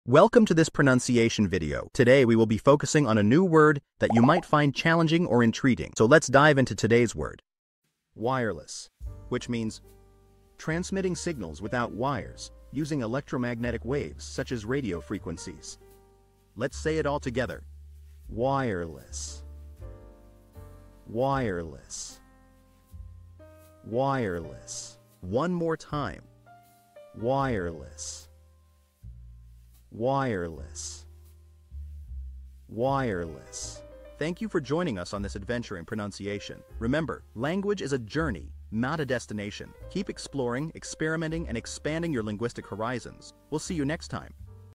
How to pronounce Wireless
Master the Pronunciation of 'Wireless' - which means : Transmitting signals without wires, using electromagnetic waves such as radio frequencies. 📝 with @PPrononciation 🎓🗣️ - [Your Guide to English]

Learn to pronounce 'Wireless' perfectly in English with our easy and engaging tutorial by @PPrononciation! 📖📽️ In this comprehensive video, we provide you with native speaker examples 🗣️👂 and phonetic breakdowns 🎵 to ensure you get the pronunciation just right.

How to say Wireless in English